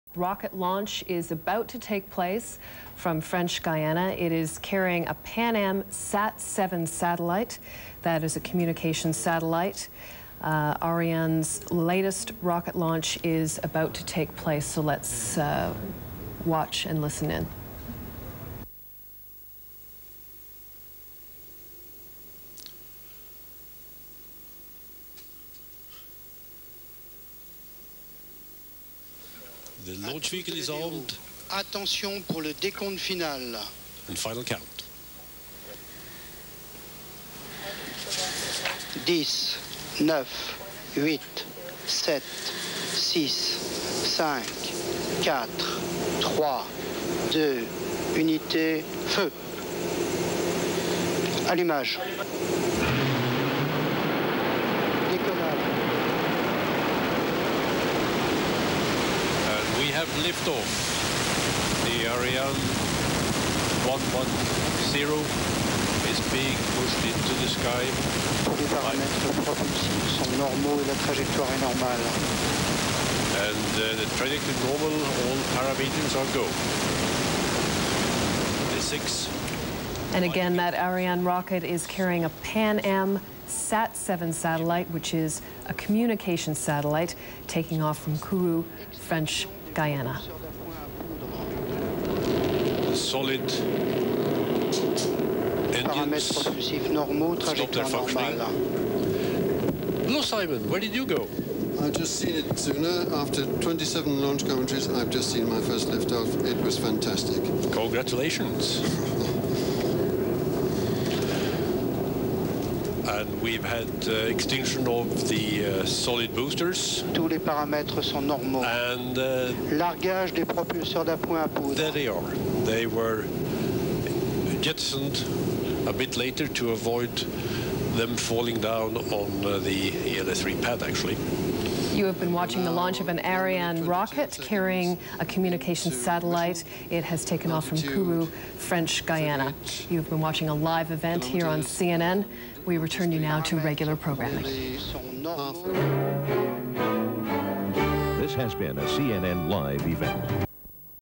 Ariane V110
Retransmission en direct du lancement sur CNN du vol V110 d'Ariane chargée de placer sur orbite le satellite de télécommunications PAS 7